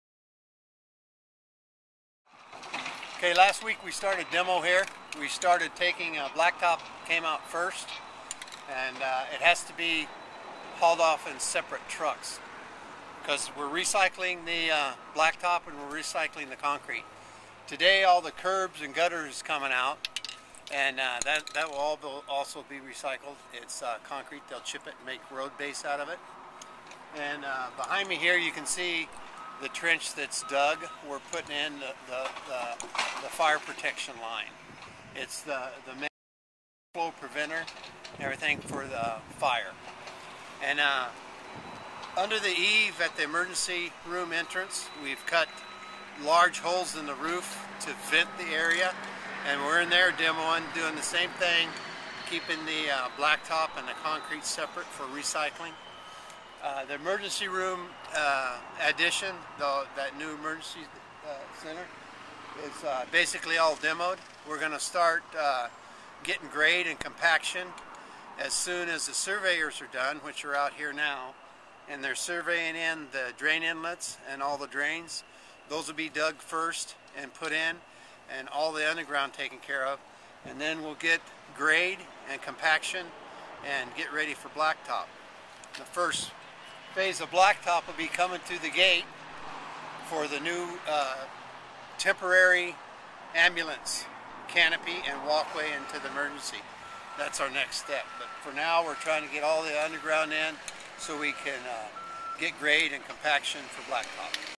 Construction Update 1 - Emergency Department Expansion
We're in the third week of construction on the Emergency Department Expansion Project. Right now we are in the demolition phase, and are working to establish a covered walkway for the temporary external entrance to the current ER. For more information about construction and access/parking during the process